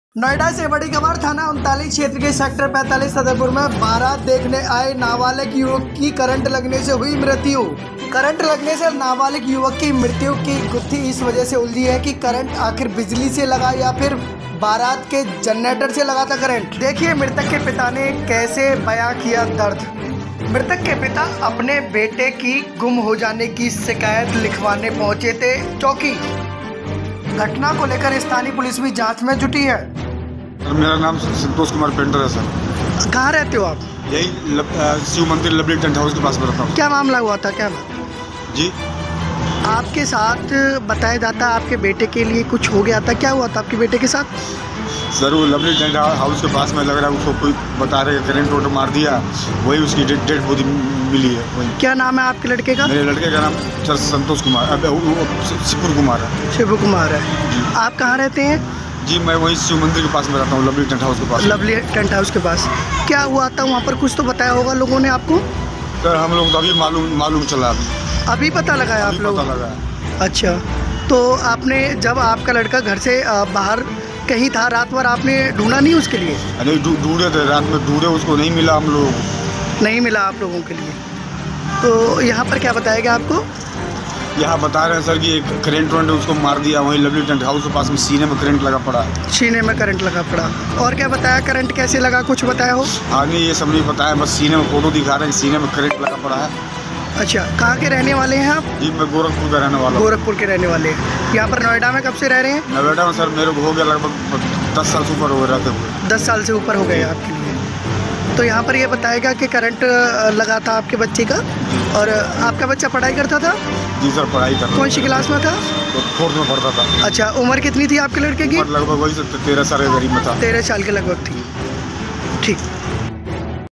बारात देखने पहुंचे नाबालिक की करंट लगने से हुई मौत#breakingnews
बारात देखने आए नाबालिक की करंट लगने से हुई मृत्यु